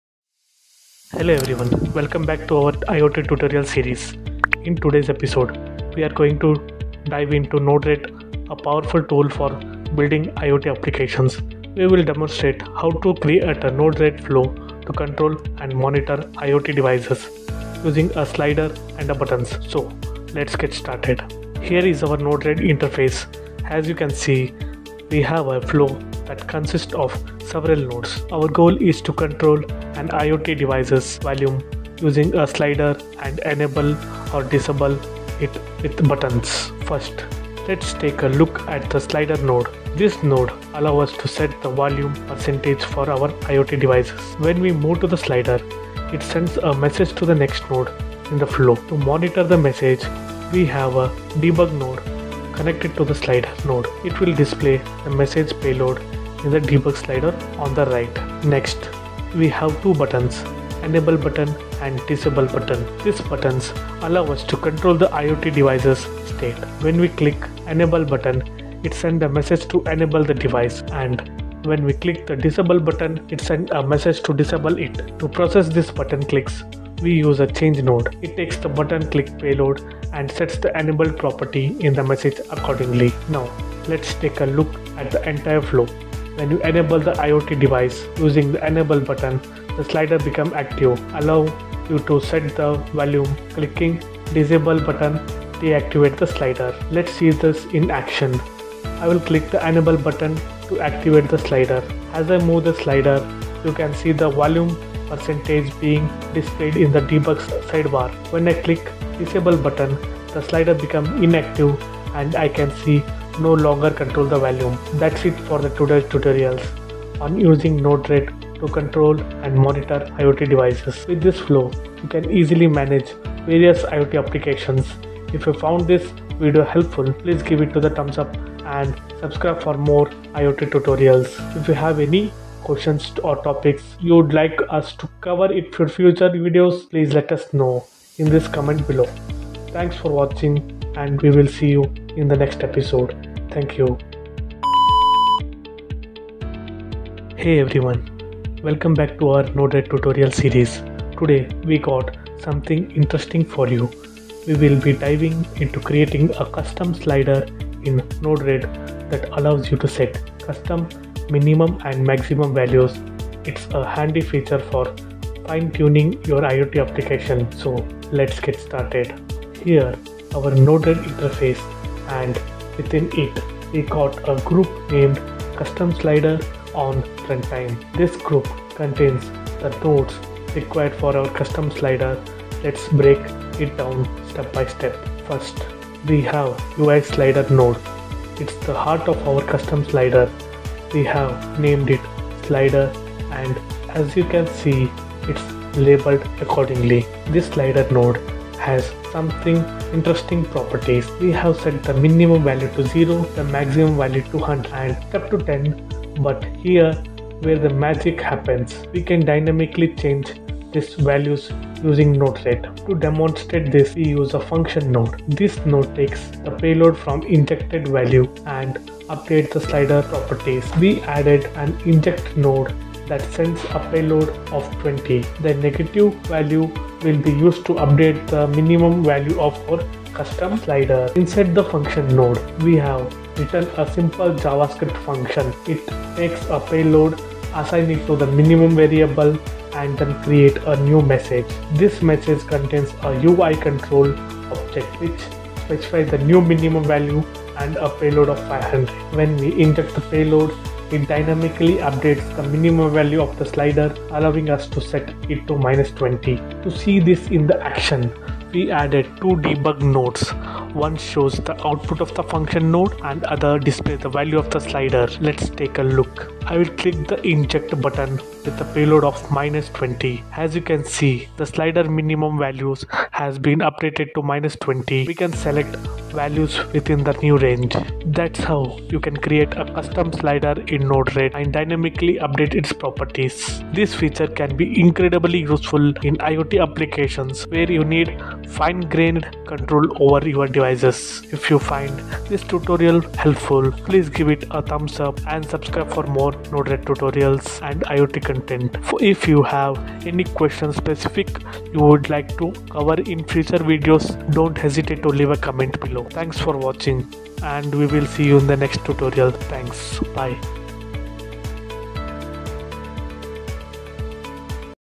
NODE-RED | IOT Controller - IOT Dashboard
Description:

**Video 1: Control and Monitor IoT Devices with Node-RED**

Discover the power of Node-RED as we create a flow to control and monitor IoT devices using sliders and buttons. Learn how to enable, disable, and adjust volume, all within a Node-RED interface.

**Video 2: Creating a Custom Slider in Node-RED**

Explore the world of custom sliders in Node-RED! We demonstrate how to dynamically modify slider properties, giving you control over minimum and maximum values. Watch as we update the slider's behavior in real-time and enhance your IoT projects.